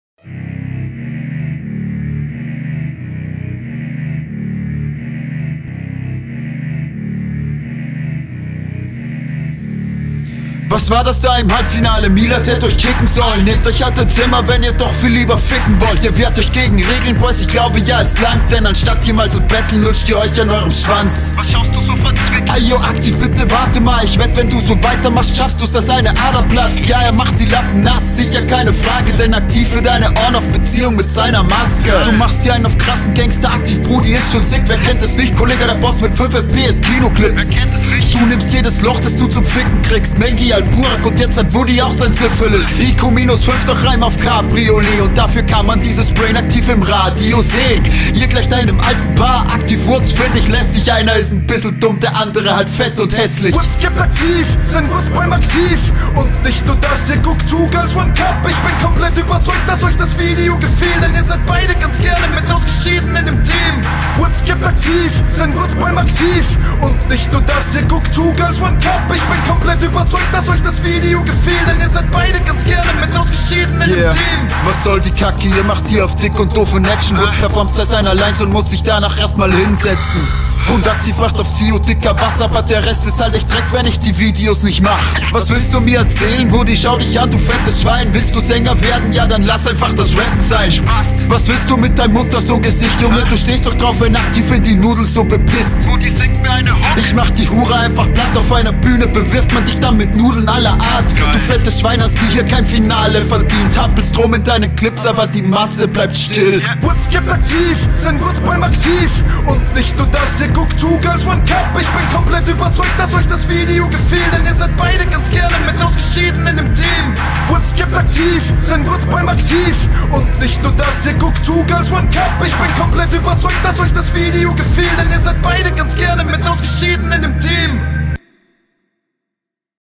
CMB 2019 | Dandy (feat. Kalkühl) vs. Aktiv x Woods (X) | FINALE
coming soon...
CMB ELITE

Beschreibung des Teilnehmers:

Text: Dandy, KalKühl
Mix/Master ist ganz sicher nicht von Aktiv :3
Cam gehalten von Kalki und mir. Tja.
Cut/Edit: ich
Beat prod. by Peat